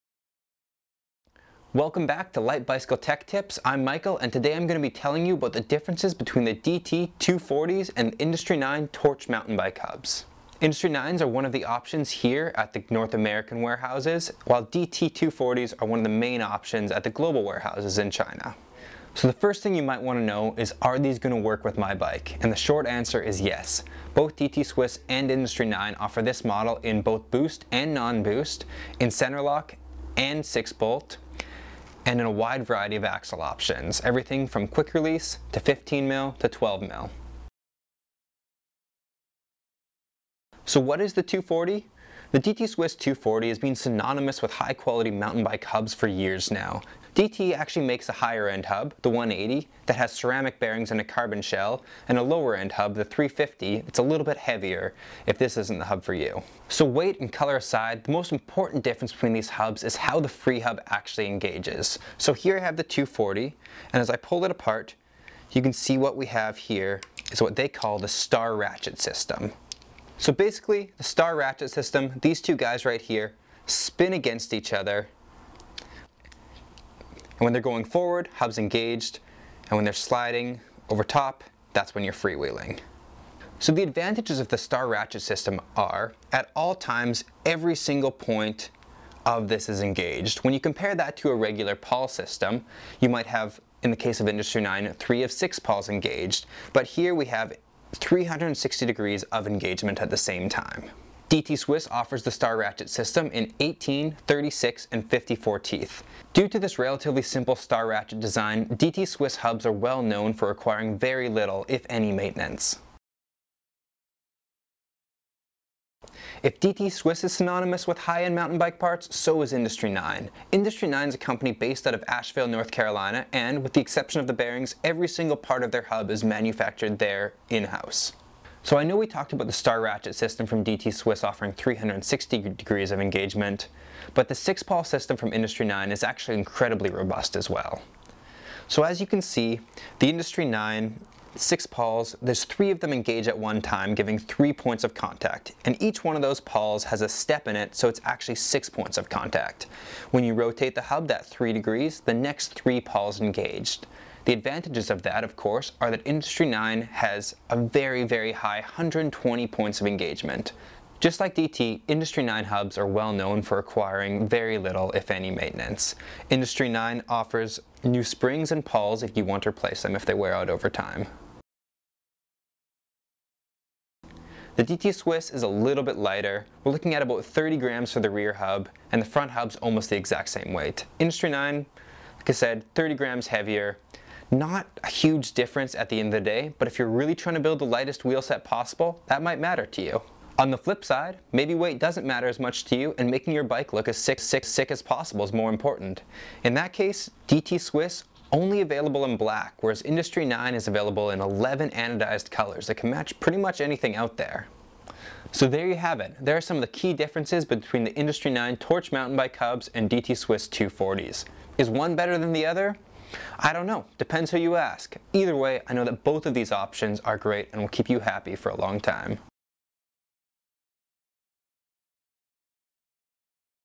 Hub Comparison: Industry 9 Torch vs DT Swiss 240
Are you wondering which hub to choose between i9 and DT Swiss? In this video, we go over the differences between these two hubs. The rest is up to you.

00:00 Introduction
00:22 Similarities
00:38 DT Swiss 240
01:54 I9 Torch
02:53 Weight

Get to Know Light Bicycle!

For More Wheel Building Suggestions